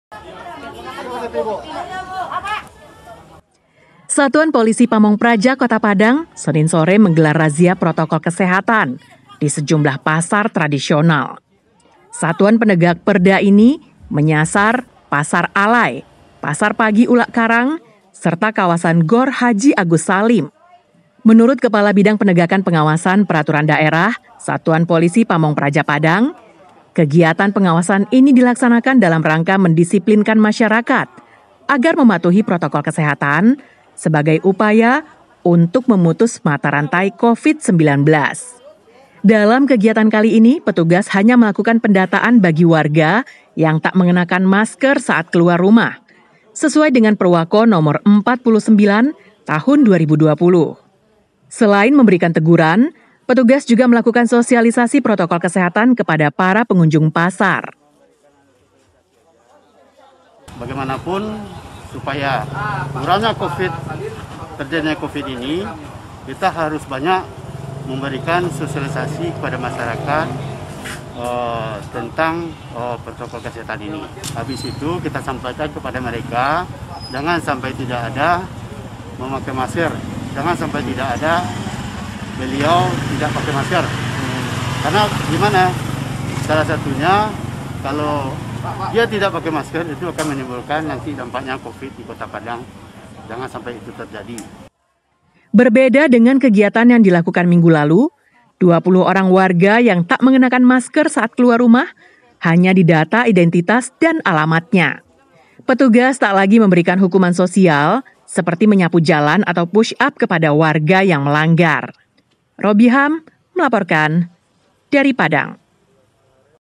PETUGAS SATPOL PP LAKUKAN SOSIALISASI PROTOKOL KESEHATAN TANPA BERIKAN TINDAKAN
WARGA YANG MELANGGAR PROTOKOL KESEHATAN HANYA DIDATA OLEH SATPOL PP PADANG